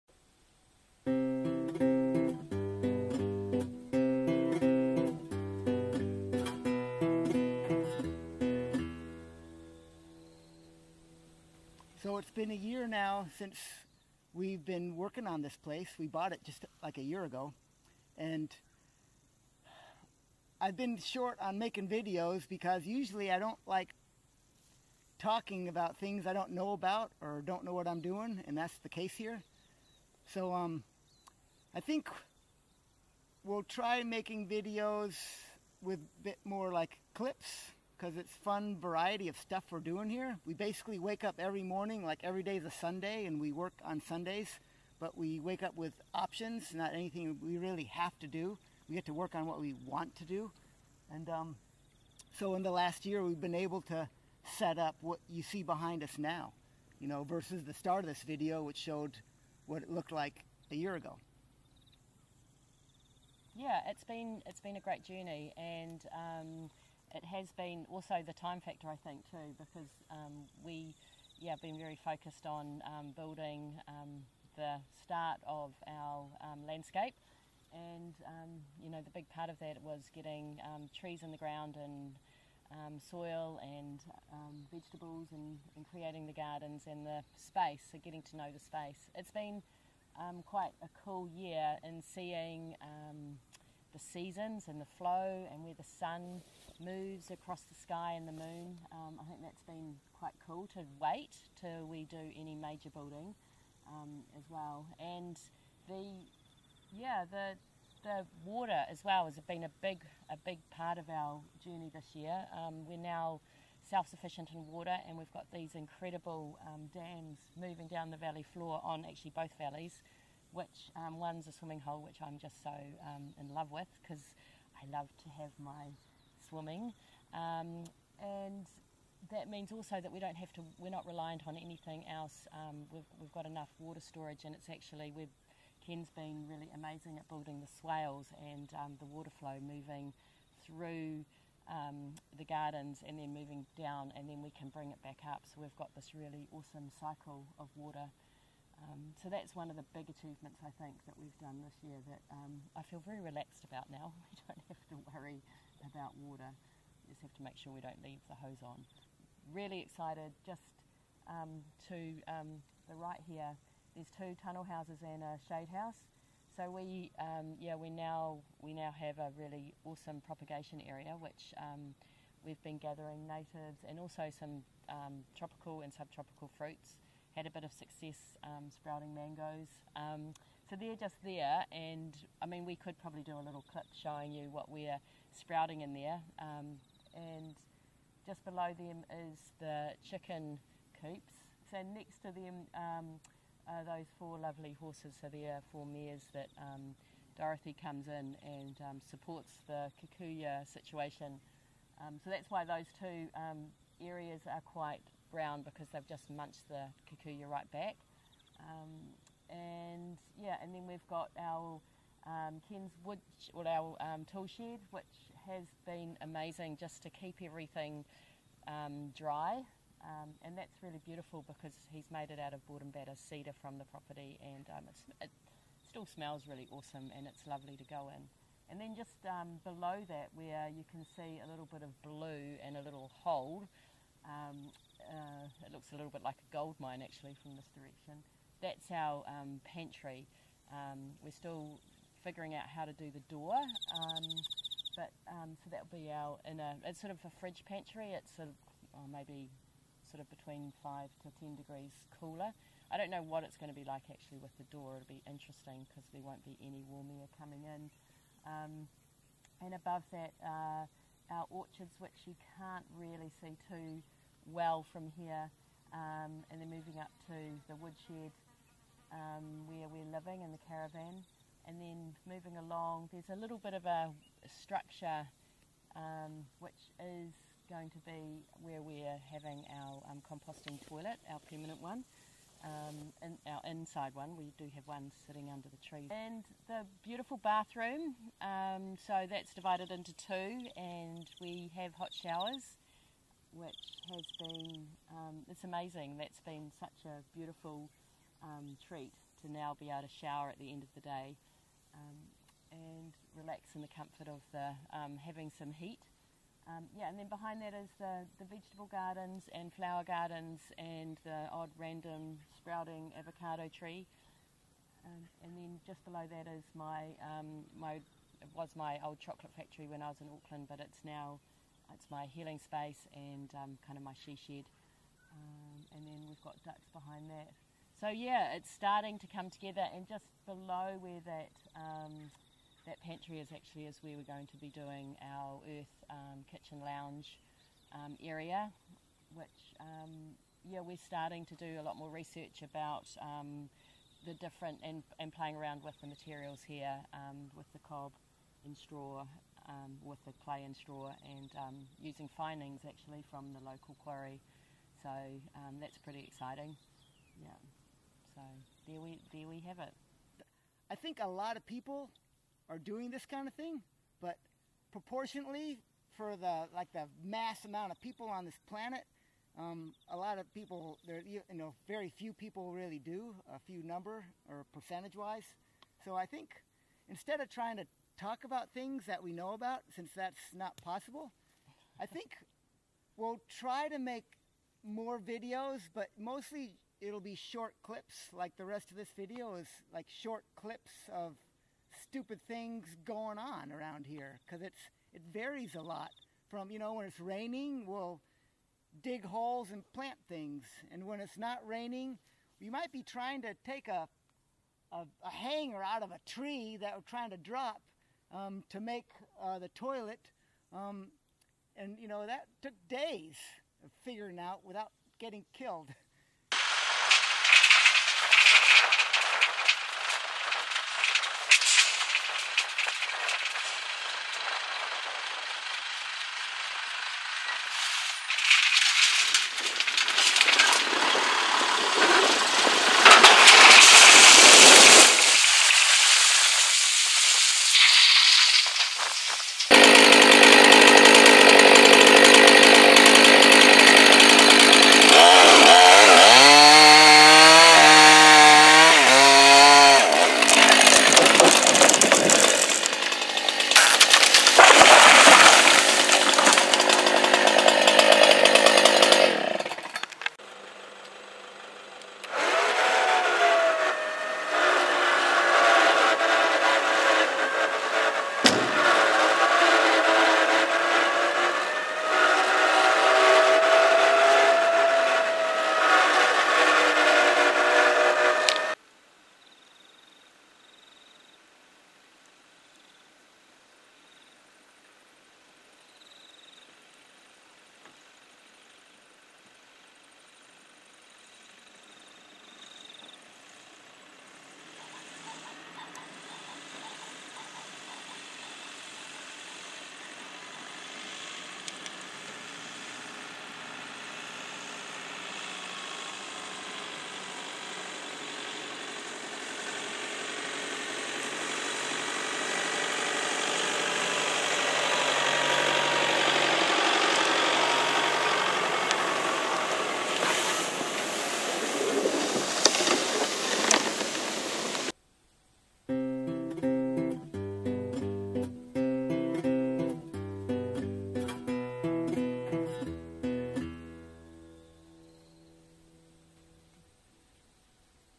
First Year Fast Forward
Ken and Chalice share a few first year projects.